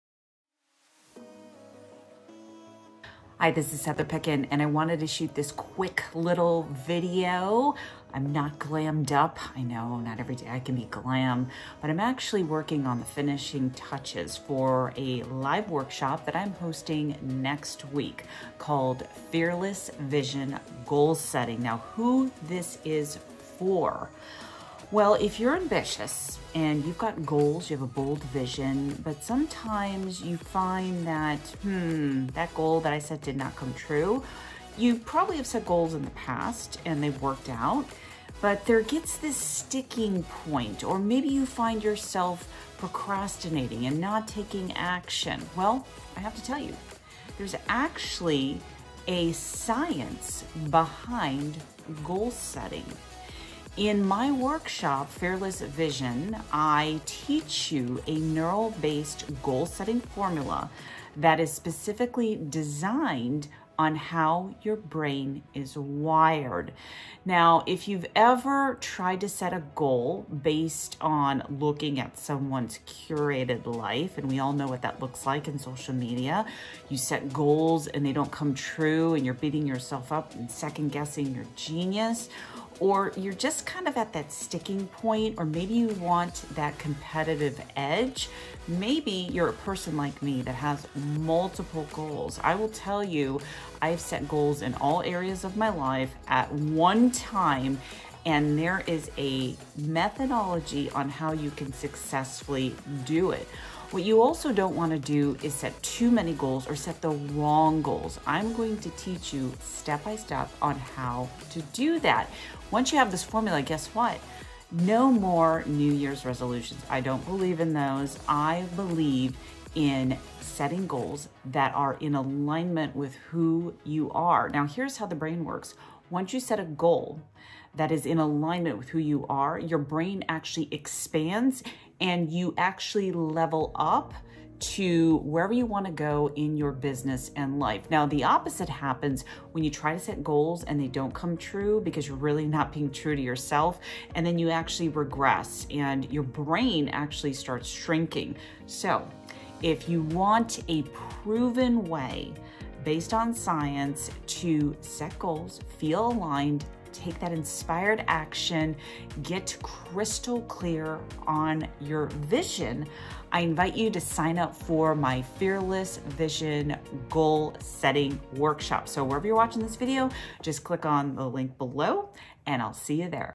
How to set GOALS Quickly in 2022: Science-Based Workshop | Plan with me
How to set GOALS in 2022: Science-Based Workshop | Plan with me

The new year is here, and our focus is now on what to expect in 2022. Regardless of whether 2021 was a banner year for you – or one of necessary change – I challenge you to do as I advise my subscribers: create the best version of the life you want in 2022

If you've tried to set goals in the past and felt like you keep coming up short, there's actually a science behind setting the right goals.

I've created a 2-hour workshop for ambitious leaders and entrepreneurs to show you how to crush your goals in 2022 and beyond.